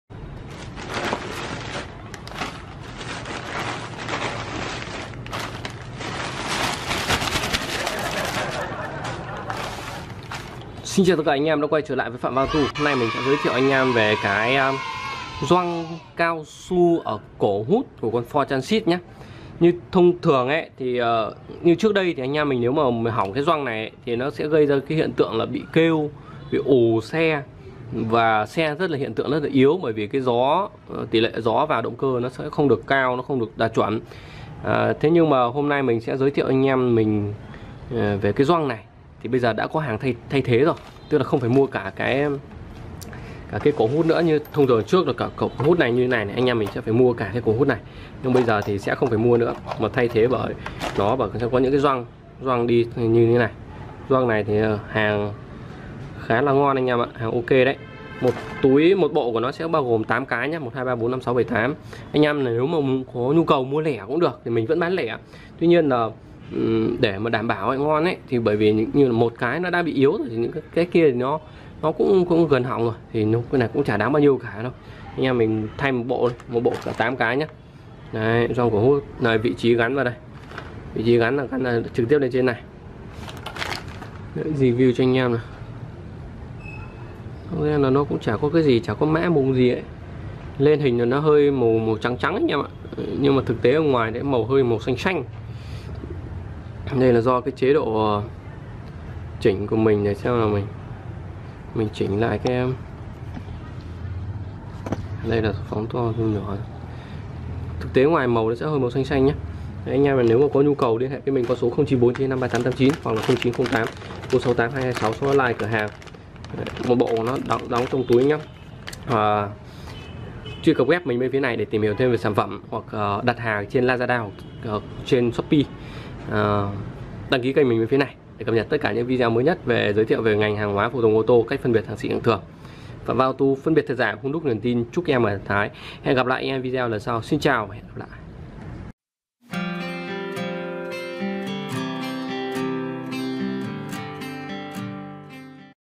Gioăng Cao Su Ford Transit - Giải Pháp Tiết Kiệm Nhất T204 | Phạm Văn auto
Đăng ký free và ấn chuông để cập nhật video khác
Cổ hút ô tô có nhiệm vụ kết nối hệ thống lấy gió từ ngoài váo trong động cơ. sau thời gian sử dụng sẽ bị hỏng lão hóa cần thay thế
Nếu Anh em đang có nhu cầu mua Gioăng_Cổ_hút_Ford_Transit để thay thế mà phân vân vì trên thị trường có quá nhiều giá và chưa biết đâu là hàng xịn, đâu là hàng nhái thì cùng Phạm_Văn_auto​​​​​​ để xem cách_phân_biệt_phụ_tùng_ô_tô_chính_hãng​​​​​​ như thế nào nhé!
Chúc Anh Em mua hàng thông thái!
Gioăng Cổ hút Ford Transit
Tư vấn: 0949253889
Mã SP : BC1Q9424AA
Hãng xe : Ford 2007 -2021
Model xe : Transit